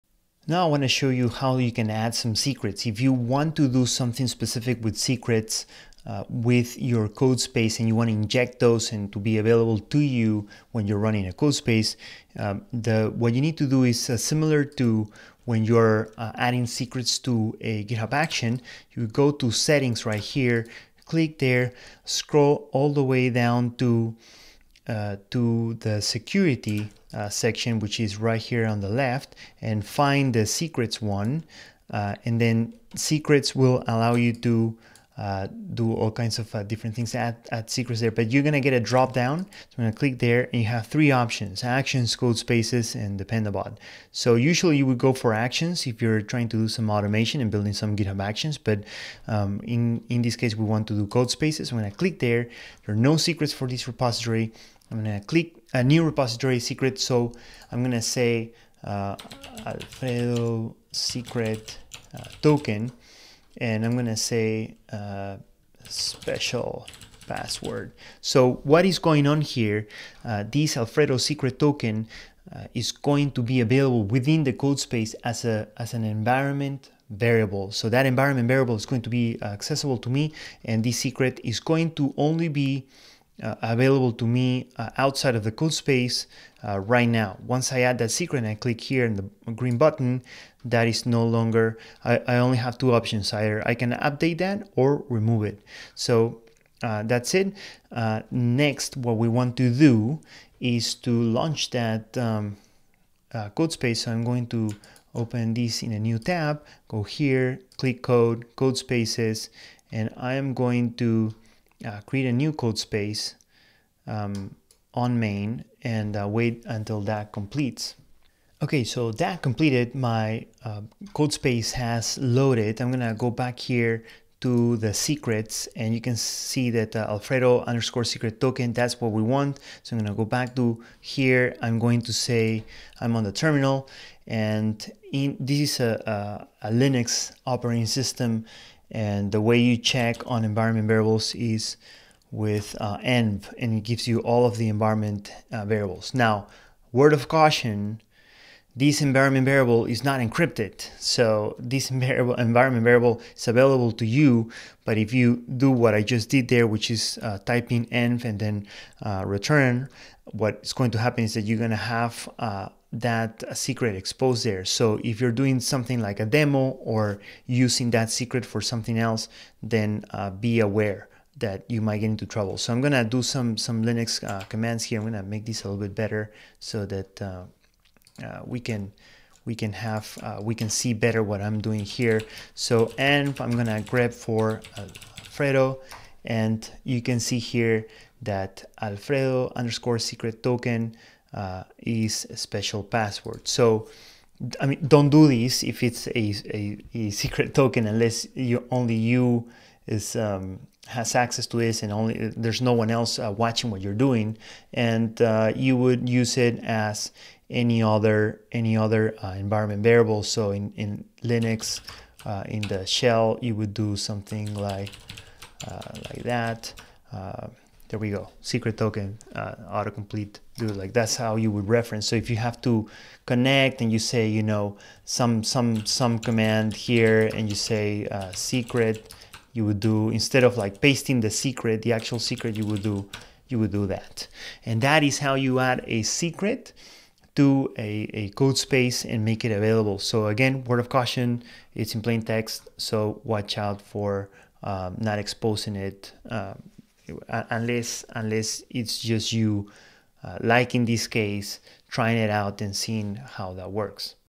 Add secrets to Codespaces
Learn how to add and configure secrets for GitHub Codespaces . These are useful if you want to have access to tokens and passwords within a Codespace instance

Resources for teachers, students, or if you want to run a workshop with example repositories: